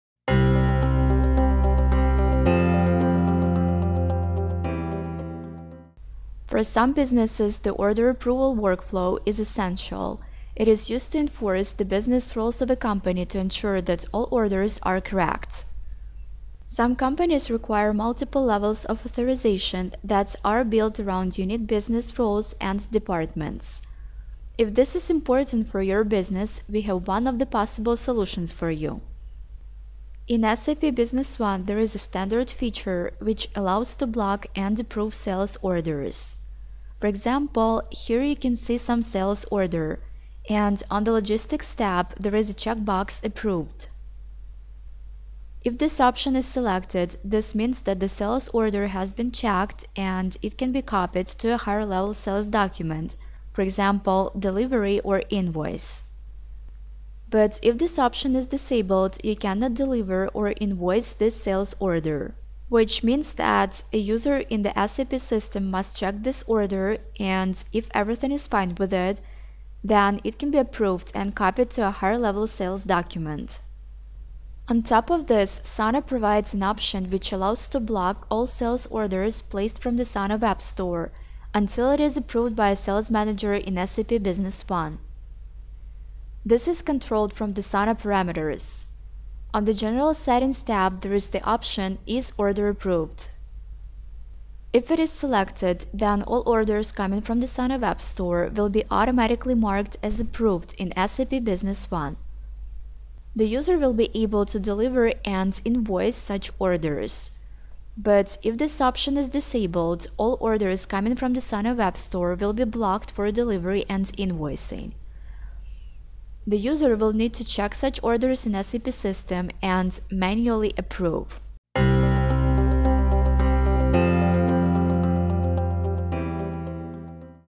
Sana Commerce – Approve Sales Order [SAP B1]
For some businesses the order approval workflow is essential. It is used to enforce the business rules of a company to ensure that all orders are correct. Some companies require multiple levels of authorization that are built around unique business rules and departments. If this is important for your business, Sana has one of the possible solutions for you.